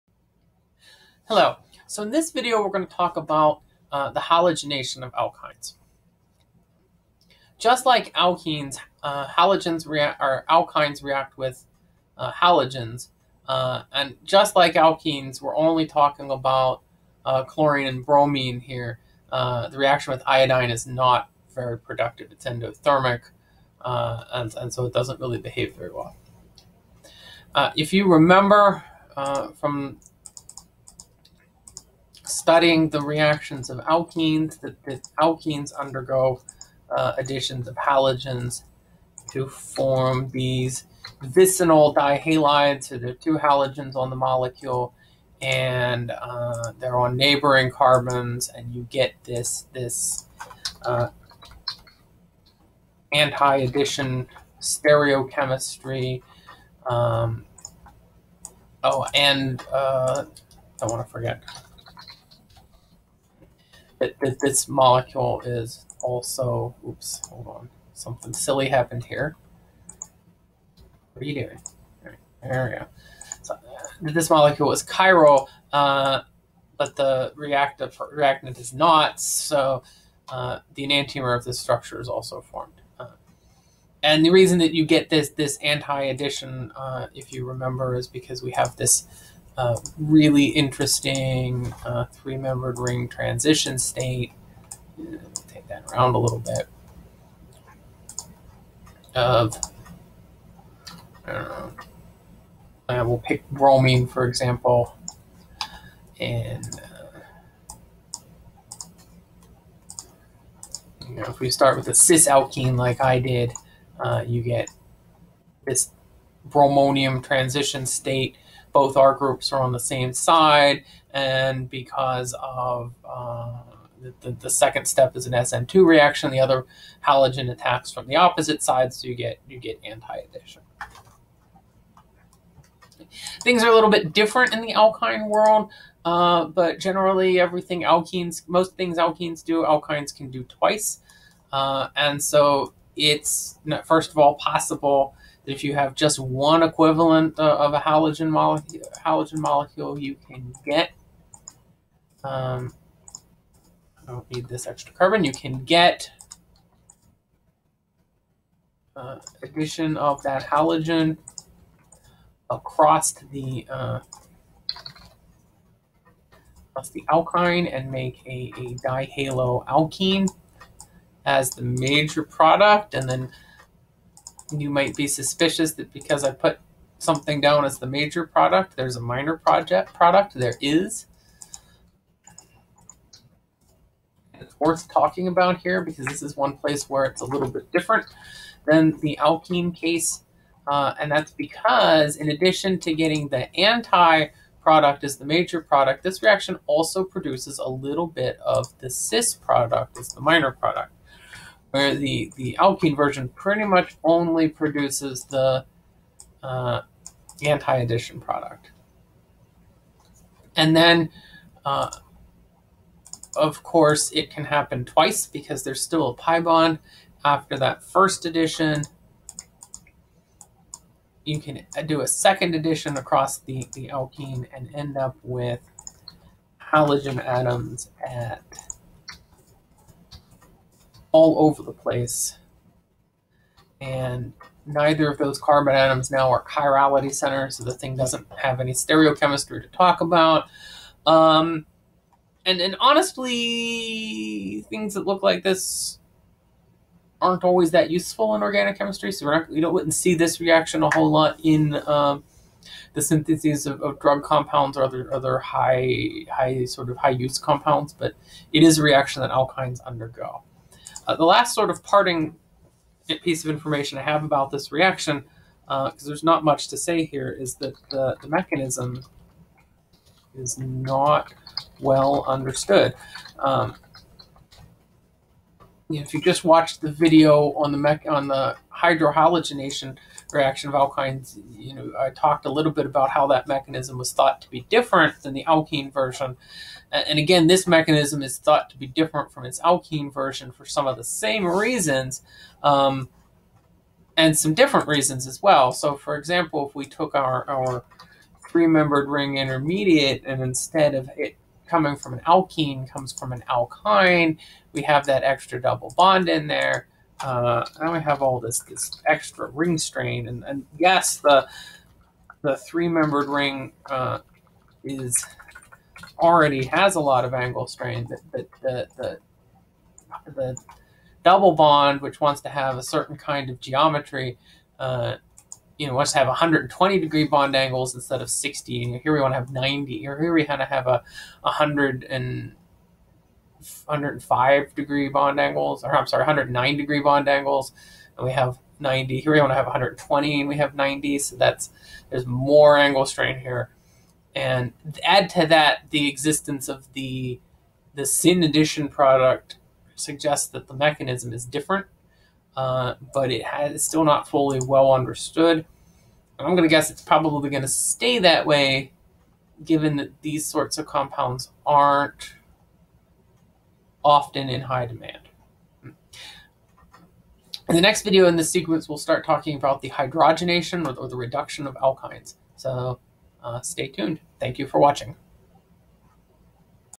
Alkyne Halogenation
Dr. Norris discusses alkyne halogenation